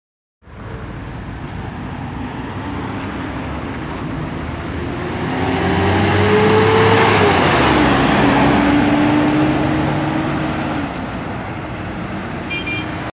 Coetc 191-Caio Mondego L Mercedes Benz O500U
Unidad Adquirida Por Raincoop Cero Kilometro En 2012 Con El Interno 101.
En 2016 Tras El Cierre De La Cooperativa, El Coche Fue Desafectado Y En 2017 Fue Adquirido Y Recuperado Y Vuelto Al Servicio Por La Cooperativa Coetc.
En Este Caso Cubriendo El Servicio De La 456 Desde Mendoza E Instrucciones Rumbo A Aduana, Ya Que De Noche Extiende Sus Servicios A Ese Destino, Porque De Día Va A Terminal Ciudadela.